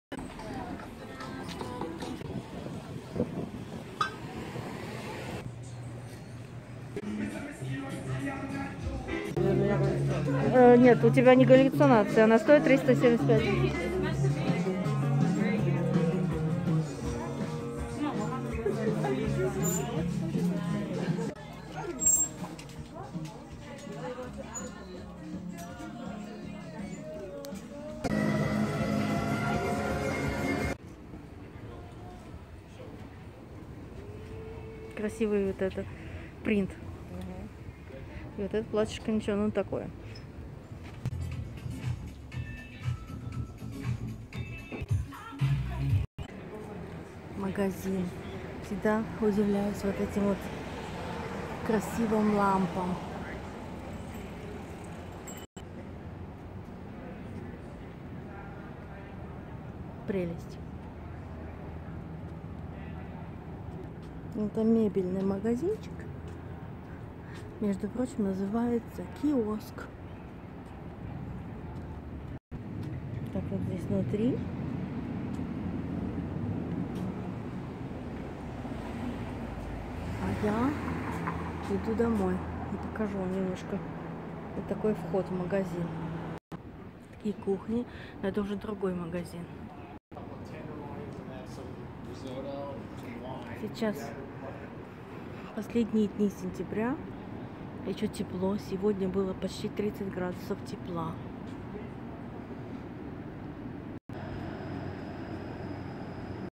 ЗАРИСОВКА: гуляем по хиппи району,блестящие блейзеры, вечер, иду домой.
ЗАРИСОВКА: гуляем по хиппи рай ону, вечер, иду домой.